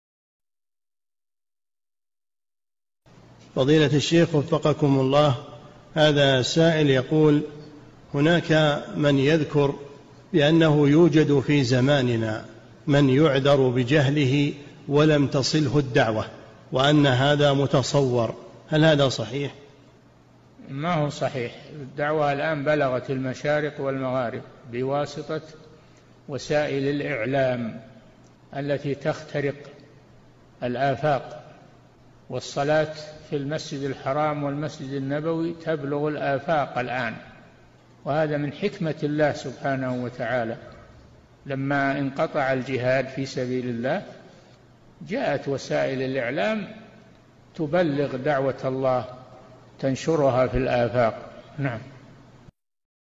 [17]Apakah Ada Udzur Bil Jahl Dizaman Sekarang? | Syaikh Shalih Fauzan Al-Fauzan Hafidzahullah
▪︎Kumpulan Fatwa Para Masyaikh Tentang Aqidah Tauhid & Syirik

~Semoga Bermanfaat 👍🏻✍🏻